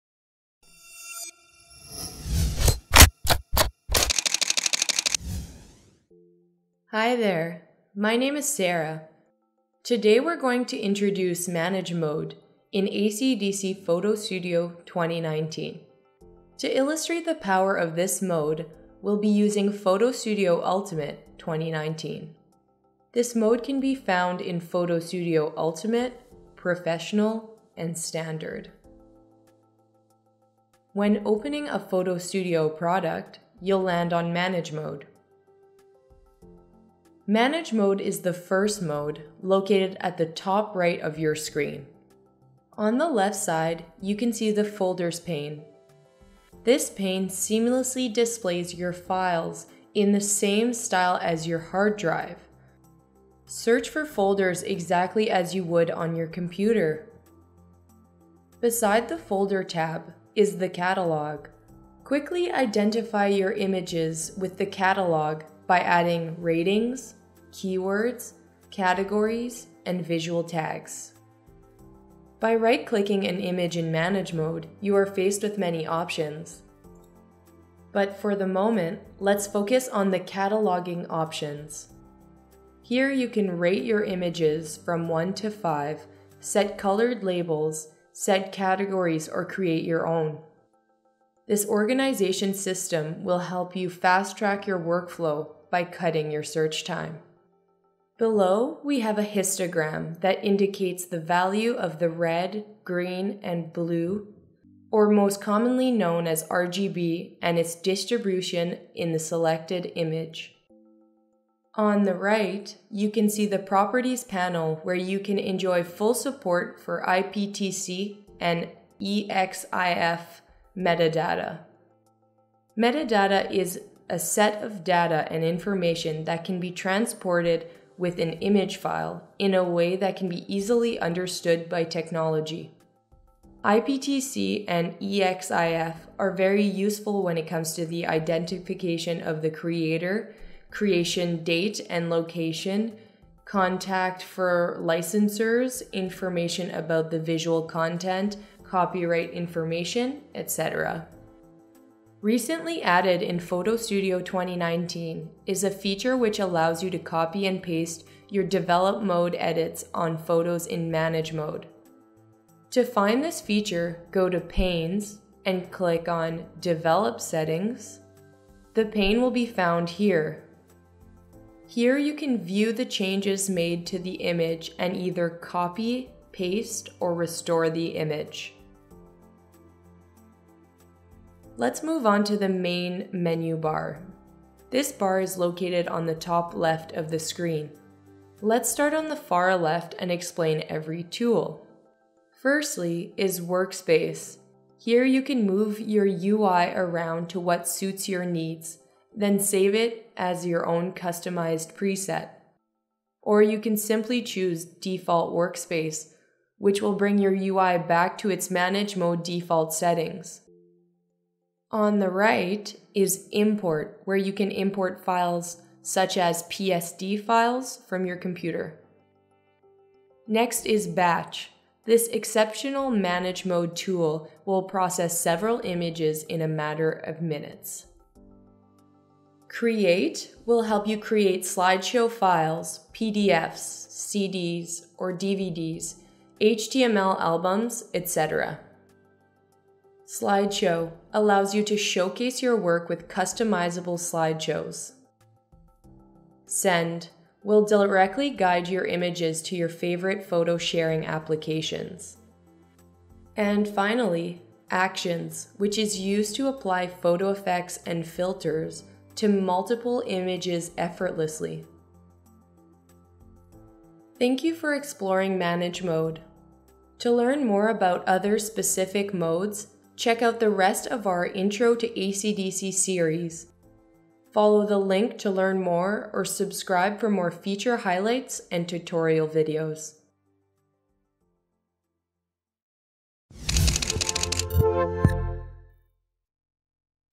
Manage - Highlight of Mode
Finding, sorting, moving, organizing, and sharing—everything you need for complete mastery over your photography workflow is right here, at your fingertips. Add ratings, hierarchical keywords, categories, and location data to your images. Quickly identify photos for further processing with visual tags or customizable color labels.